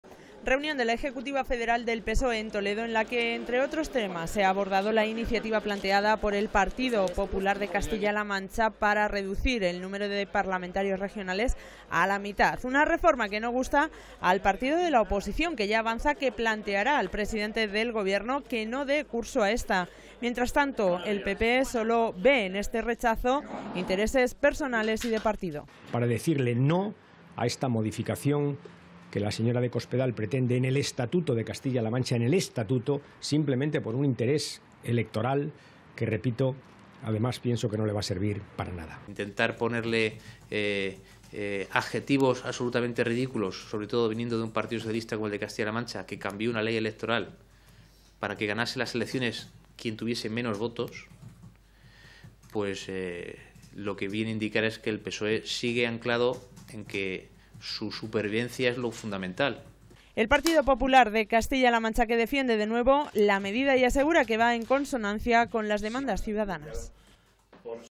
Hoy reunión de la Comisión de la Ejecutiva Federal del PSOE en Toledo
Hoy reunión de la Comisión de la Ejecutiva Federal del PSOE en Toledo y la reforma del estatuto de Castilla- La Mancha que llega esta semana al congreso de los diputados y que ha sido el tema que ha centrado hoy el debate político de la región.
La reforma de la Ley Electoral de Castilla- La Mancha en el debate político de la región. Esta mañana se ha reunido la Ejecutiva Federal del PSOE en Toledo donde su Secretario General ha avanzado que pedirá al Presidente del Gobierno que paralice la iniciativa cuando llegue al congreso. Mientras tanto desde el PP castellano-manchego defiende la medida y  asegura que los Socialistas la rechazan solo por intereses personales y partidistas.
Reunión de la Ejecutiva Federal del PSOE en Toledo en la que entre otros temas se ha abordado la iniciativa planteada por el PP de reducción del número de parlamentarios regionales a la mitad. Una reforma que no gusta al partido de la oposición que ya avanza que planteará al Presidente de Gobierno que no de curso a esta. Mientras tanto el PP solo ve en este rechazo intereses personales y de partido.
El PP que defiende de nuevo la medida y asegura que va en consonancia con las demandas ciudadanas.